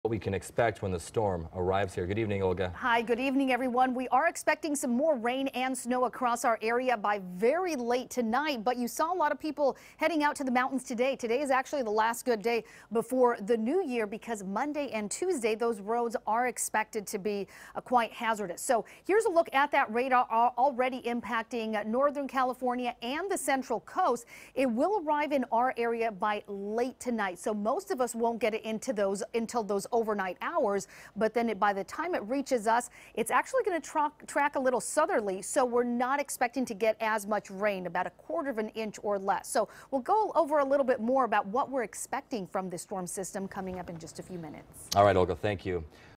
Olga's Forecast (Dec. 29)
The latest update on the rain and storm here in the Southland.